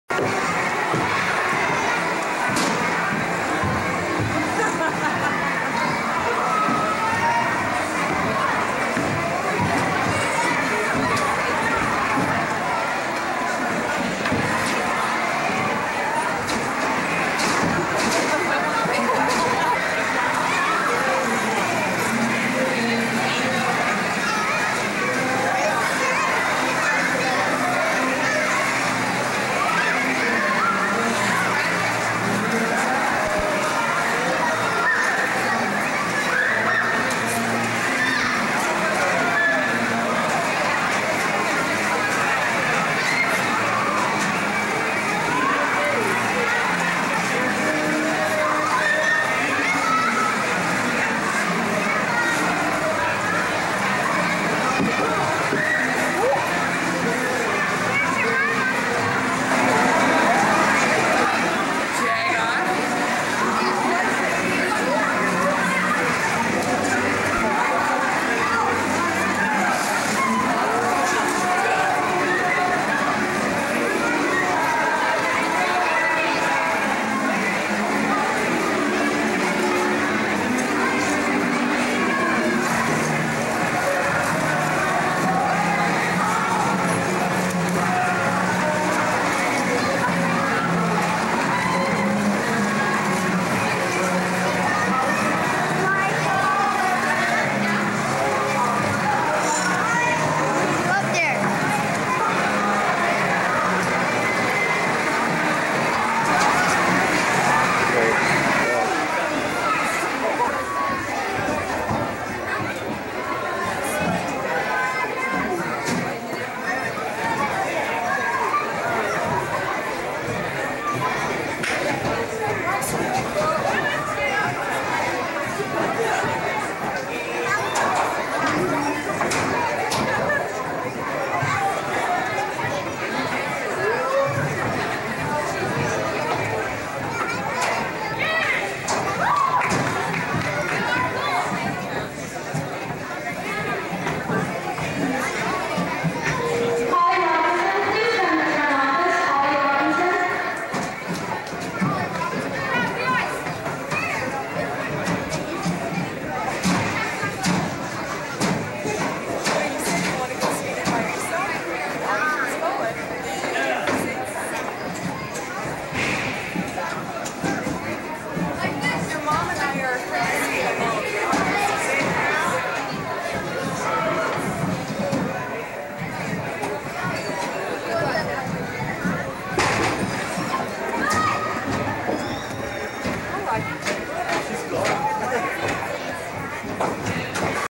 KaylaOnIce
Kayla's first experience with ice skates at about age 4?